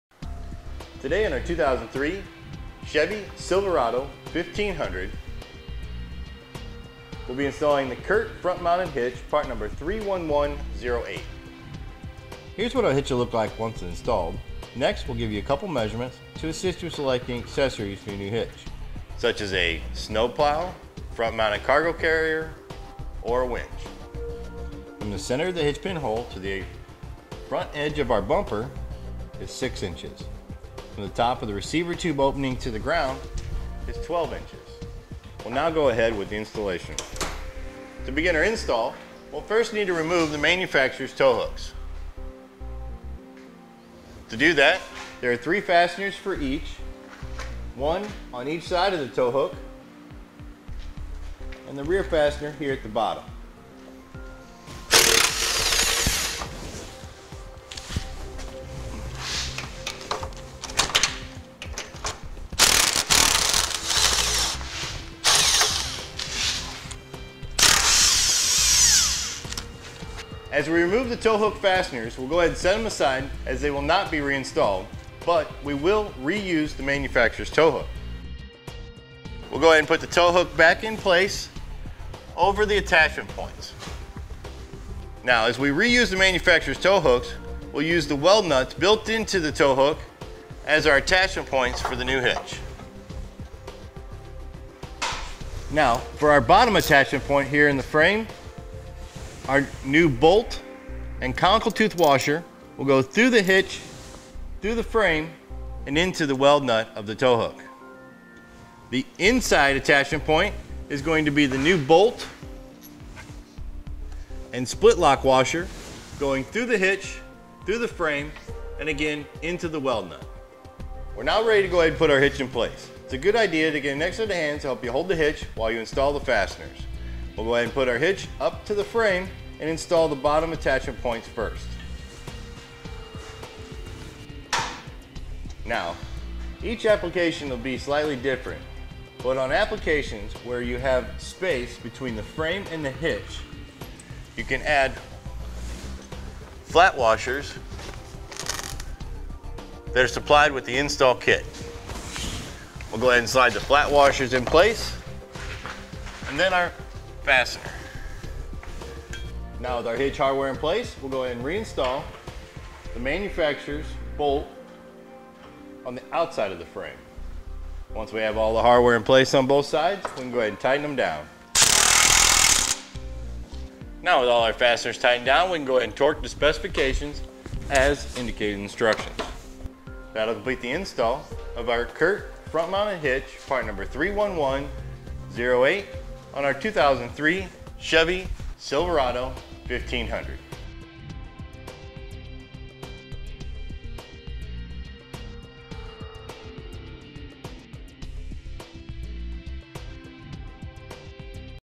etrailer | DIY Install: Curt Front Mount Trailer Hitch Receiver on your 2003 Chevrolet Silverado
Today on our 2003 Chevy Silverado 1500, we'll be installing the Curt Front Mounted Hitch, part number 31108. Here's what our hitch will look like once installed. Next, we'll give you a couple of measurements to assist you in selecting accessories for your new hitch, such as a snowplow, front mounted cargo carrier, or a wench. From the center of the hitch pin hole to the front edge of our bumper is 6 inches. From the top of the receiver tube opening to the ground is 12 inches. We'll now go ahead with the installation. To begin our install, we'll first need to remove the manufacturer's tow hooks. To do that, there are 3 fasteners for each, one on each side of the tow hook, and the rear fastener here at the bottom. As we remove the tow hook fasteners, we'll go ahead and set them aside as they will not be reinstalled, but we will reuse the manufacturer's tow hook. We'll go ahead and put the tow hook back in place over the attachment points.

As we reuse the manufacturer's tow hooks we'll use the weld nuts built into the tow hook as our attachment points for the new hitch. For our bottom attachment point here in the frame, our new bolt and conkle tooth washer will go through the hitch, through the frame and into the weld nut of the tow hook. The inside attachment point is going to be the new bolt and split lock washer, going through the hitch, through the frame, and again into the weld nut. We're now ready to go ahead and put our hitch in place. It's a good idea to get an extra set of hands to help you hold the hitch while you install the fasteners. We'll go ahead and put our hitch up to the frame and install the bottom attachment points first. Each application will be slightly different.

On applications where you have space between the frame and the hitch, you can add flat washers. They're supplied with the install kit. We'll go ahead and slide the flat washers in place, and then our fastener. Now with our hitch hardware in place, we'll go ahead and reinstall the manufacturer's bolt on the outside of the frame. Once we have all the hardware in place on both sides, we can go ahead and tighten them down. With all our fasteners tightened down, we can go ahead and torque to specifications, as indicated in the instructions. That will complete the install of our Curt Front Mounted Hitch, part number 31108 on our 2003 Chevy Silverado 1500. .